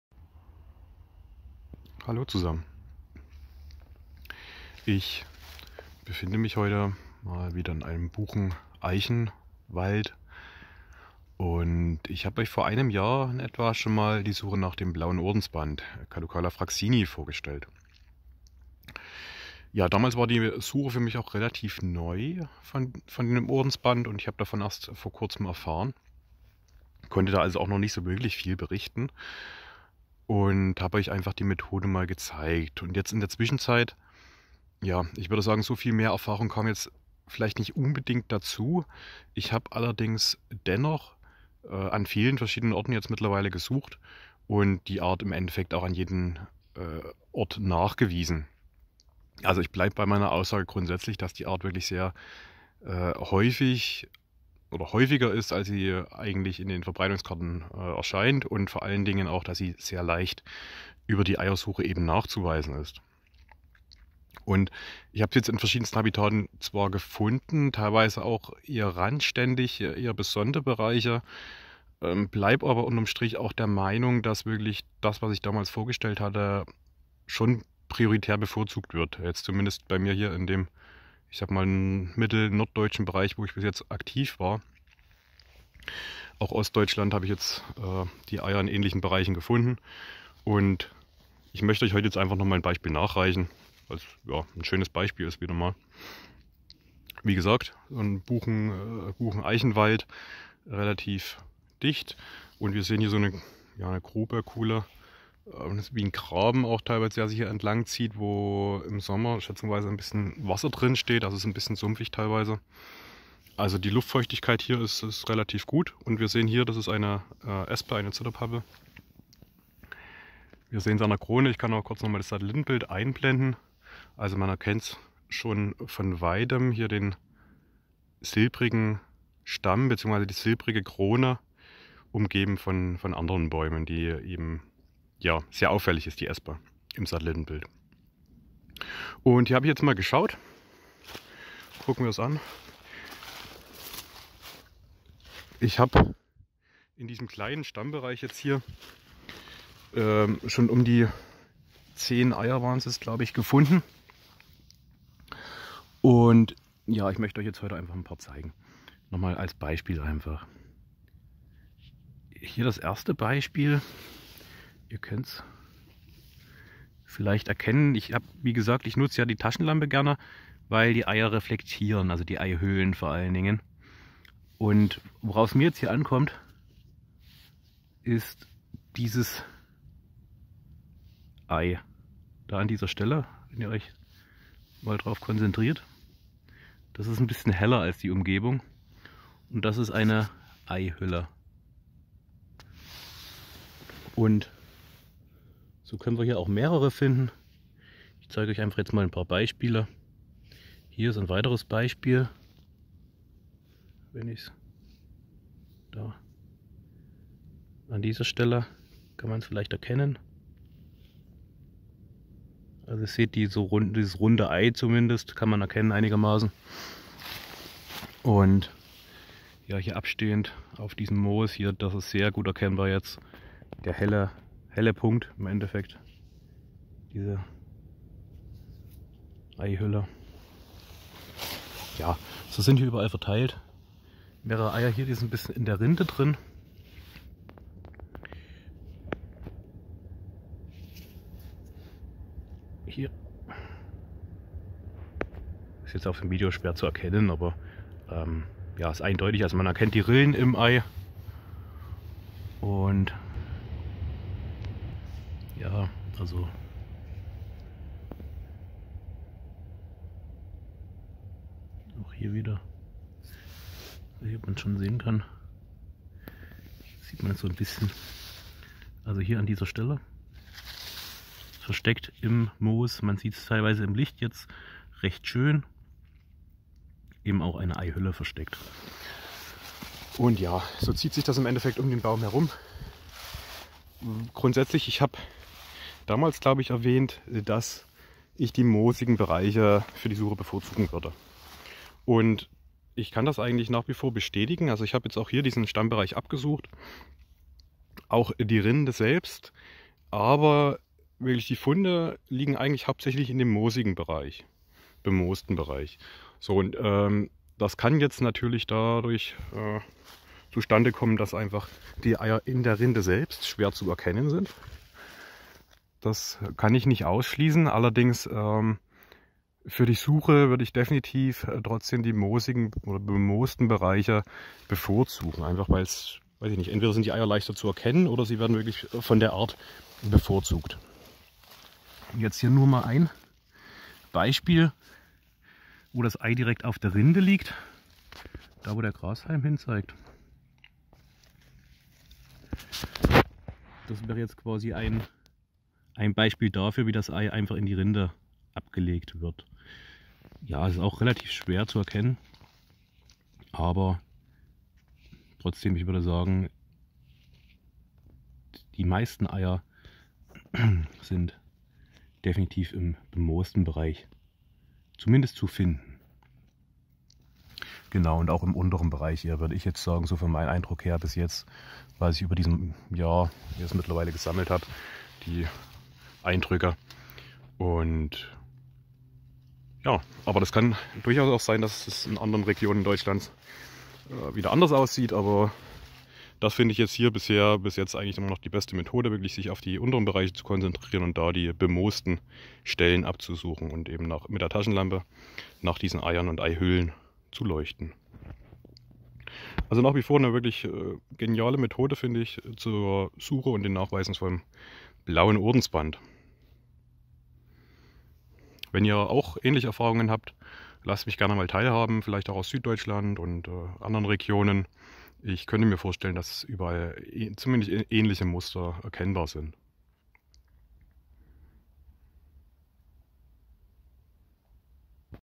Der Nachweis des Blauen Ordensbands (Catocala fraxini) - Folge II
Vor einem Jahr präsentierte ich euch schon einmal die Suche nach dem Blauen Ordensband. In den vergangenen Monaten konnte ich die Art in vielen Regionen und den verschiedensten Stellen nachweisen. Heute möchte ich euch hierzu noch einmal ein typisches Beispiel präsentieren.

Kapitel:
0:00 Einführung
1:36 Das Habitat
2:00 Die Espe im Satellitenbild
2:21 Konkrete Beispiele